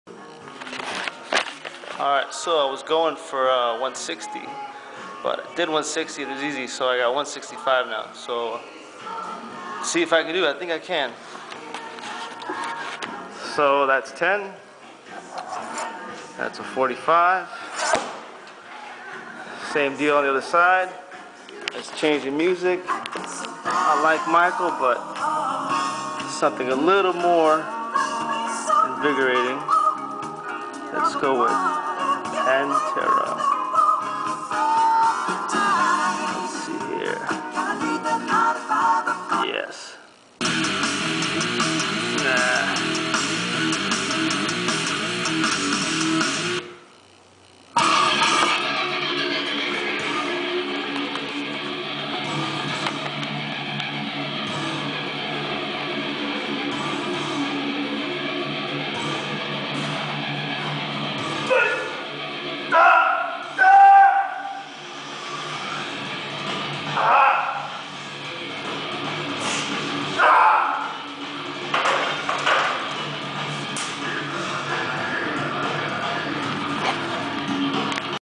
Strict Press (OHP) 155lb x 1 @ 164lb BW
Epic arithmetic fail. I thought I hit a PR then as I was uploading the video I realized m math was way off.